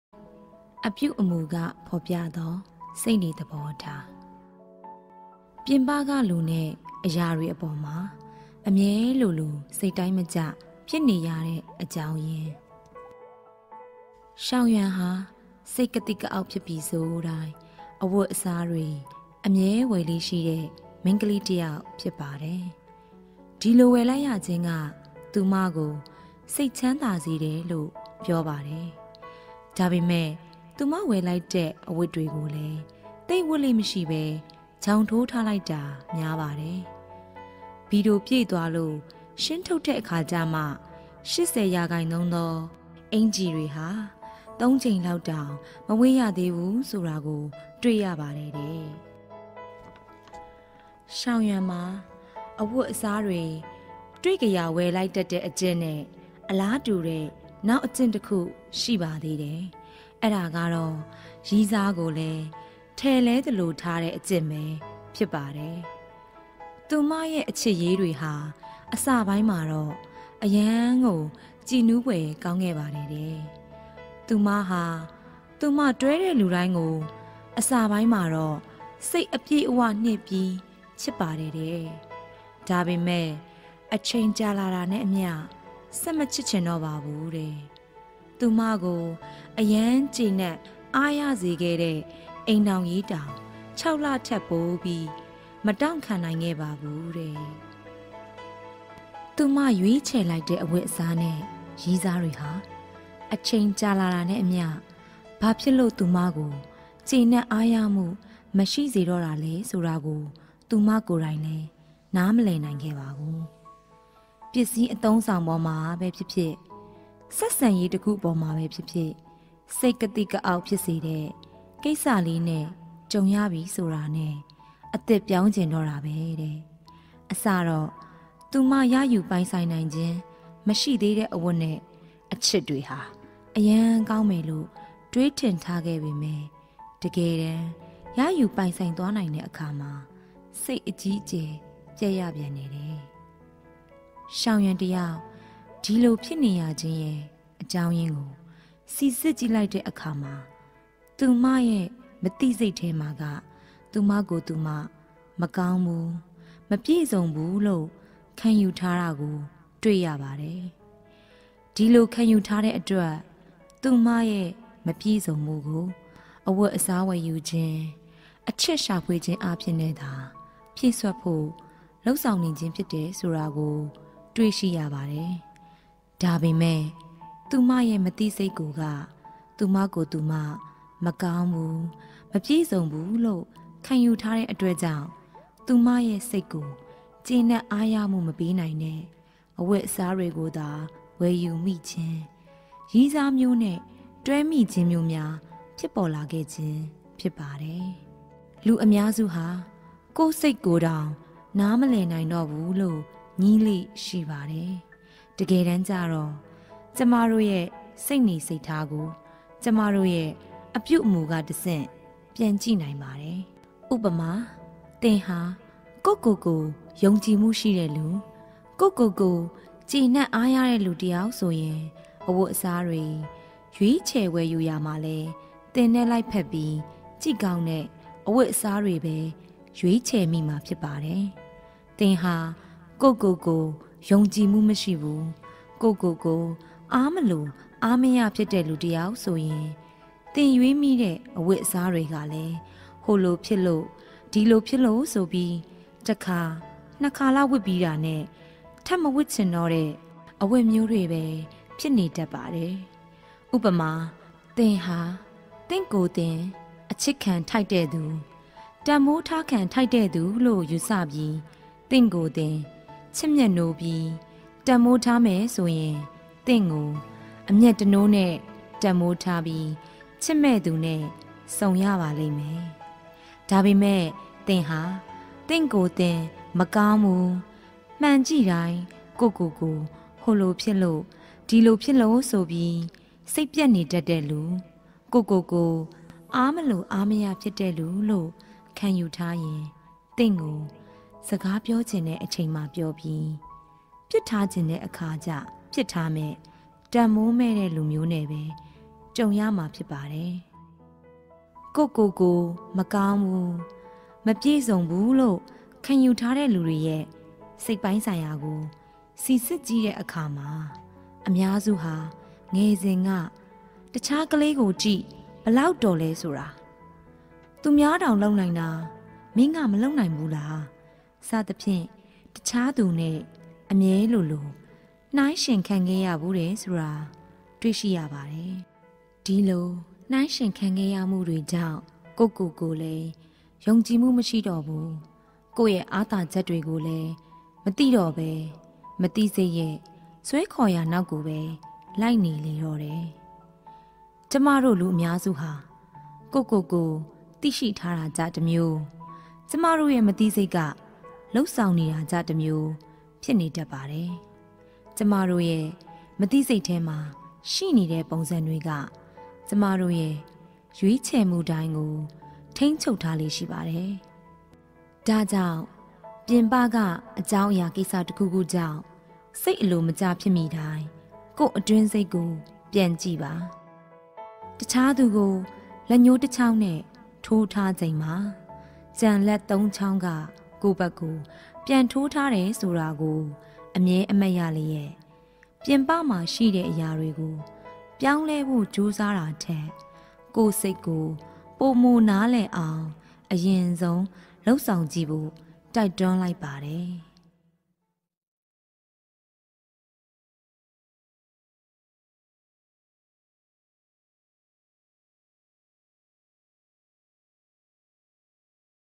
ပြင်ပကလူနဲ့အရာတွေအပေါ်မှာ အမြဲလိုလို စိတ်တိုင်းမကျ ဖြစ်နေရတဲ့ အကြောင်းရင်း..
အချိန်တစ်ခု၌ရပ်တန့်နေခဲ့ကြသူများ စာအုပ်၏အခန်း (၄) ကို မျှဝေထားခြင်း ဖြစ်သည်။